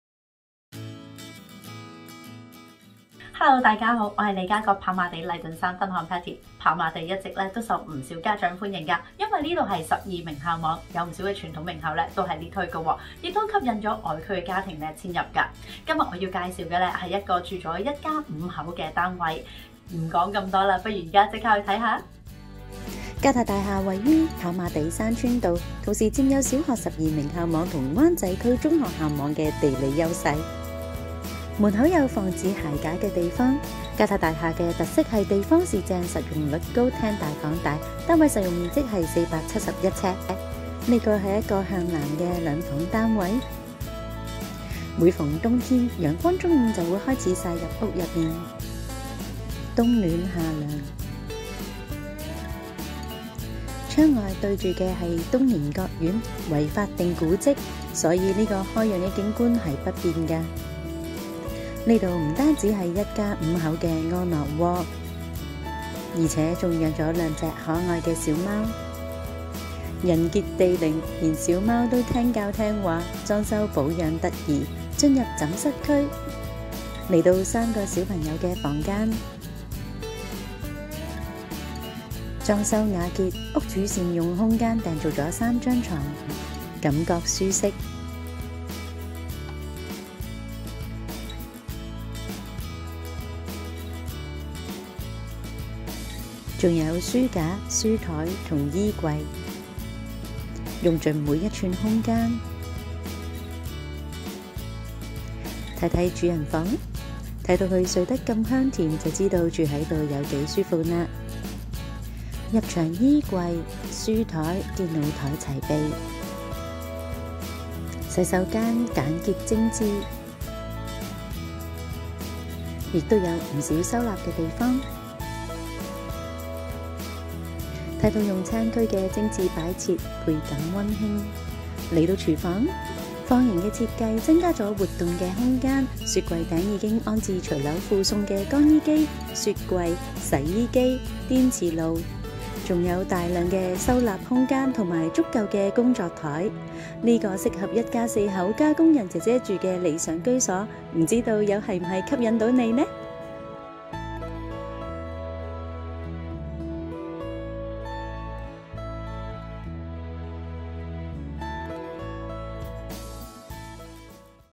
【KOL睇真盤】 Patty Tandon 筍盤推介：嘉泰大廈 中層 D室
KOL睇真盤 – Patty Tandon 筍盤推介：
跑馬地
物業地址：嘉泰大廈 中層 D室（物業編號: BU12234167）

間隔 ：2房 1厠
賣點 ：上車首選, 八成按揭, 小學12校網, 中學灣仔區名校網